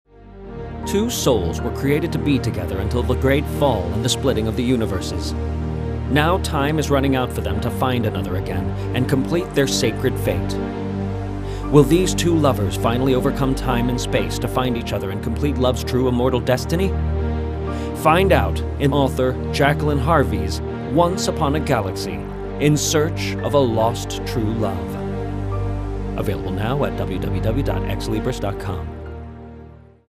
Once Upon a Galaxy by Jacqueline Harvey - 30 sec
When you reach the twilight of your existence, look back on your life and realize you have accomplished all your desires; you have to share your fulfillment with the world, your children, your family, your dearest friends, and everyone who contributed to your well-being. If the saying "Practice makes perfect" goes, I hope it applies to me. This is my second attempt at writing a book, a sequel to my first one. I do not consider myself a literary writer, far from it. As a matter of fact, in my school years, not only couldn't I put two and two together, but composition was my second weakest subject. As English is not my mother language, it's a double challenge. I have many times used dictionaries to prove my point. At an early age, playing with my dolls, I would tell them all kinds of stories, so you can say that I am more of a storyteller. This is my fairy tale as close to the truth as can be told however, to protect the anonymity of the characters, their names have been changed. Enjoy!